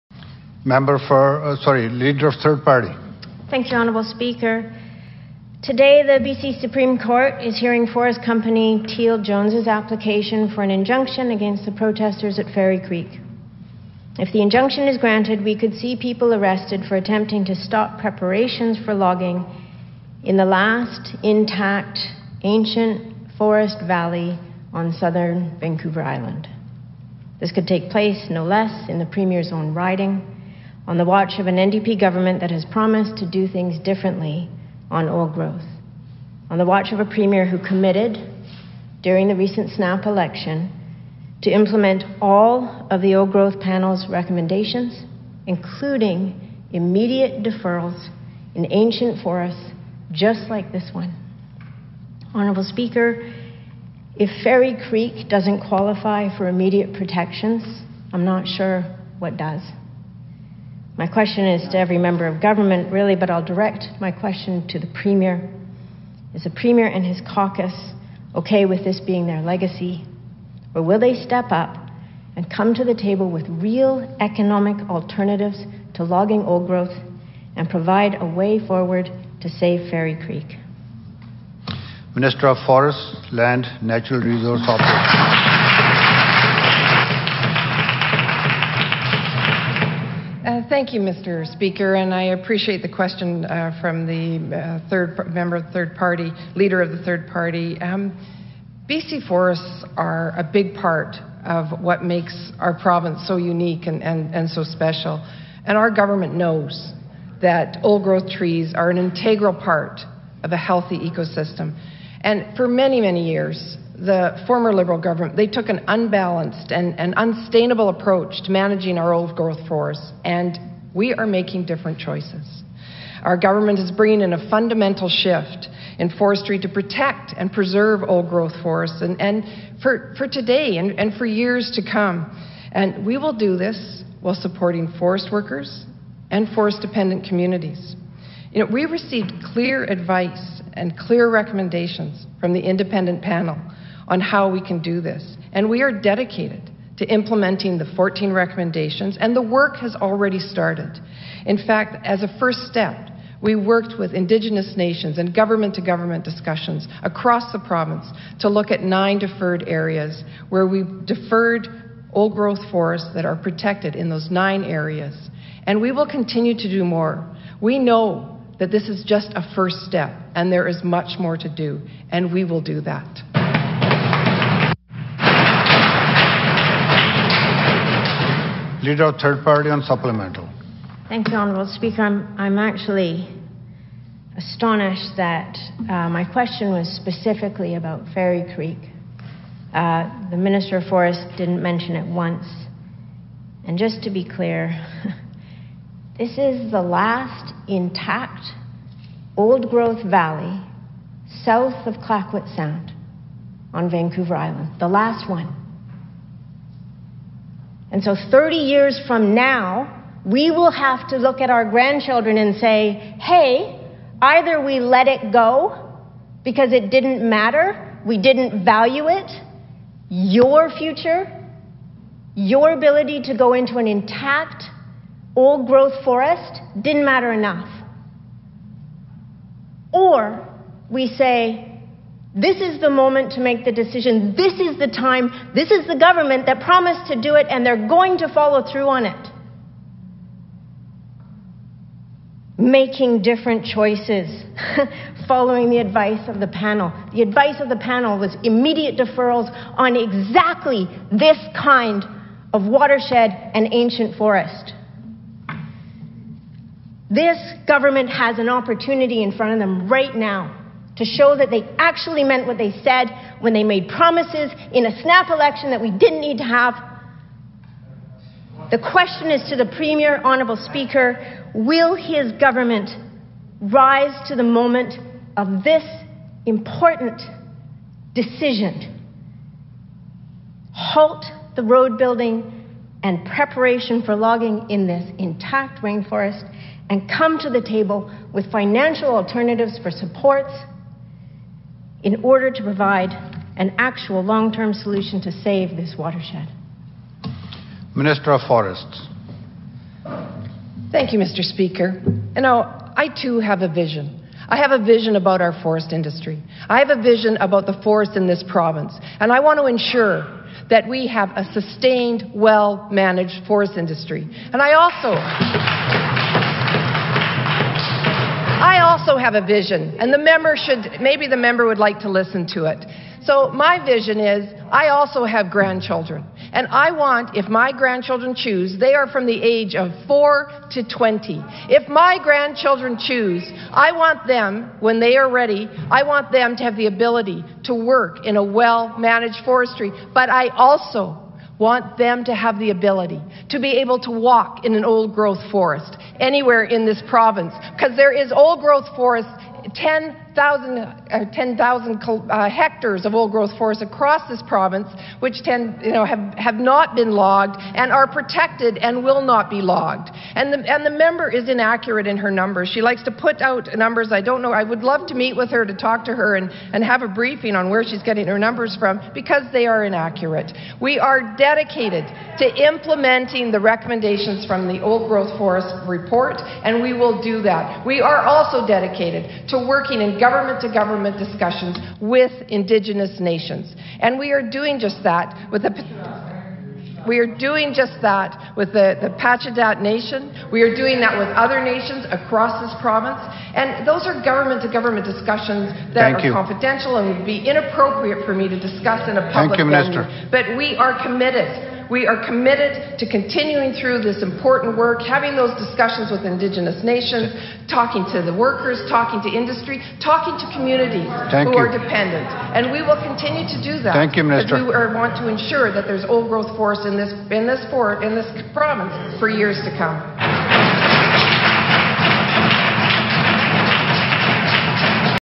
VIDEO - A Passionate call in the BC Legislature to save Fairy Creek old growth
This is the Hansard video record of Green Party leader Sonia Furstenau standing in the British Columbia legislature on March 25 during question period to make a passionate call for the provincial government to save the old-growth forest of Fairy Creek.

For more information about the fight to save old-growth forests